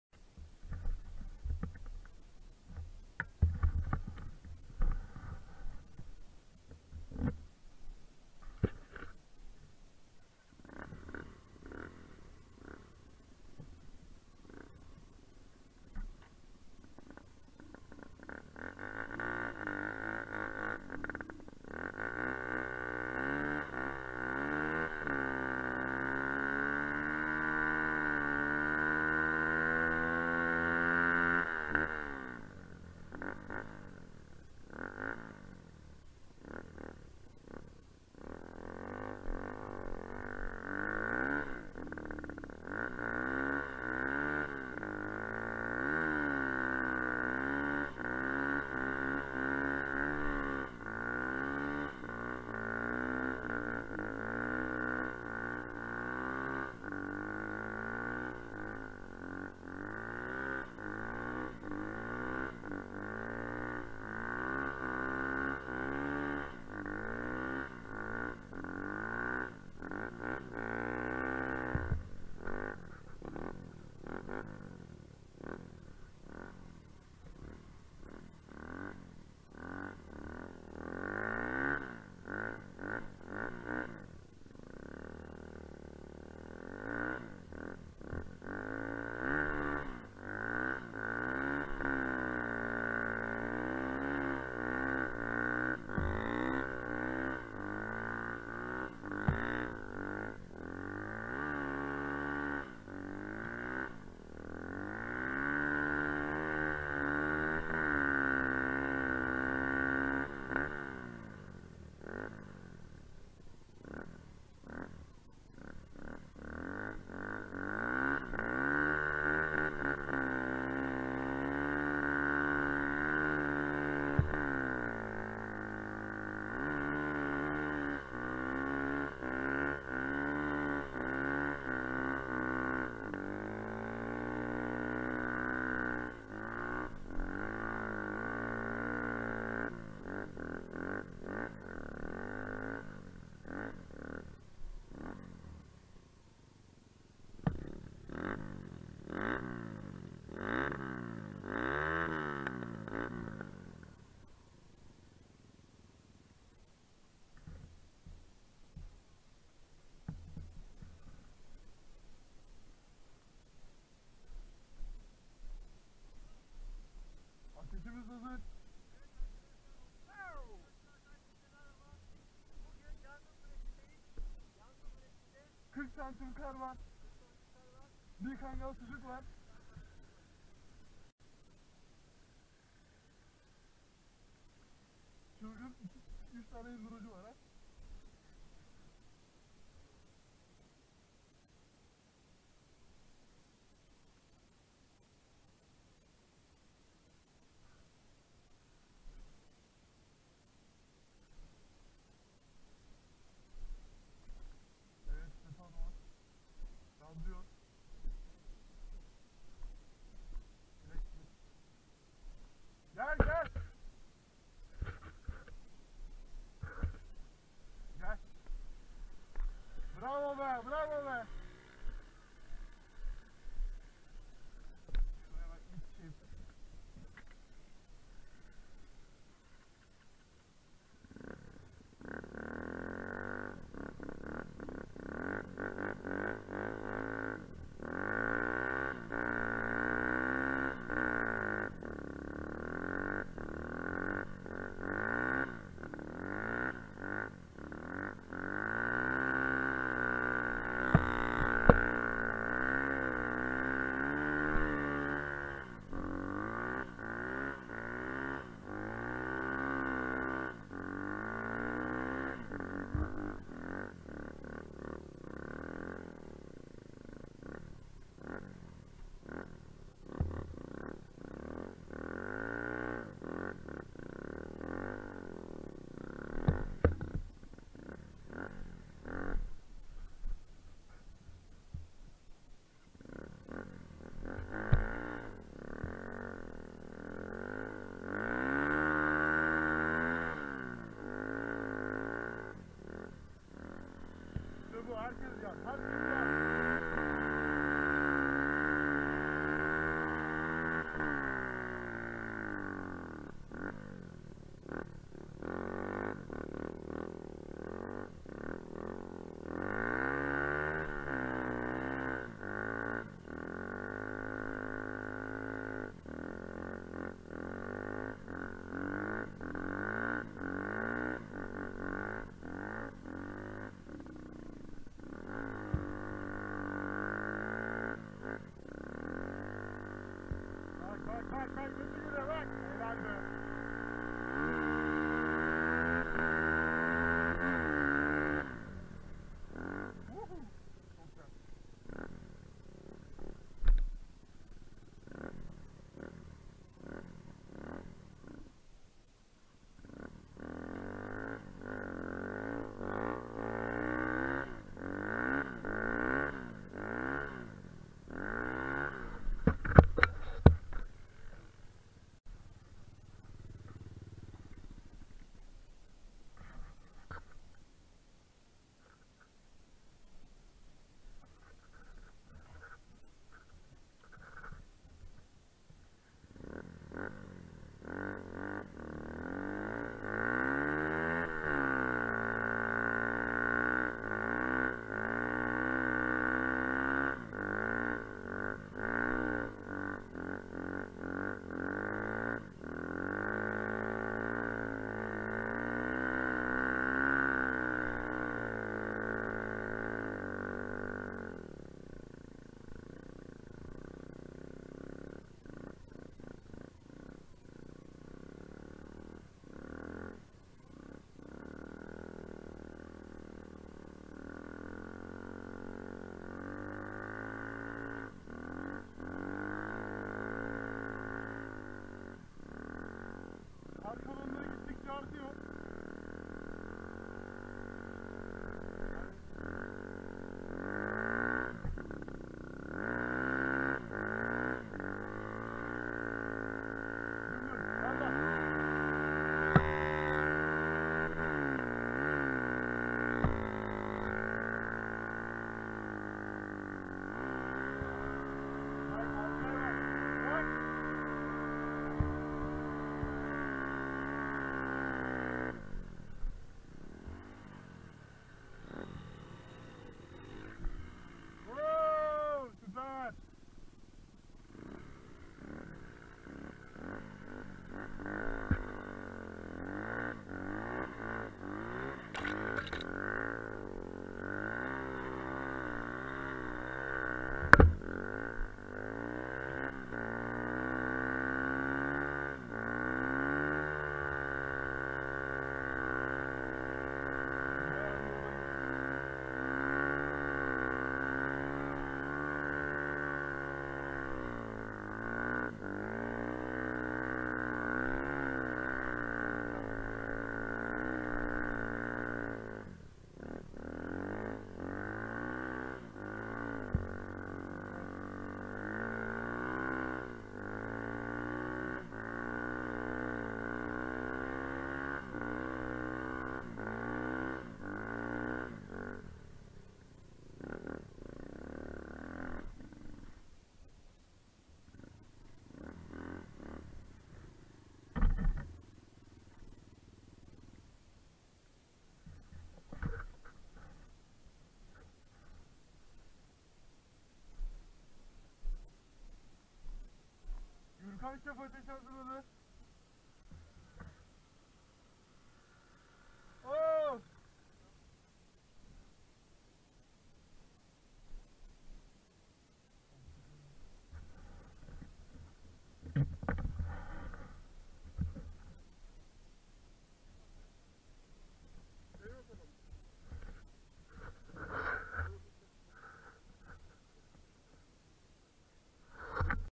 OSMANELİ KAR KIŞ ENDURO SÜRÜŞÜ
OSMANELİ ENDUROCULAR OLARAK BU SEFER KAR DA MOTOR SÜRÜYORUZ. İYİ SEYİRLER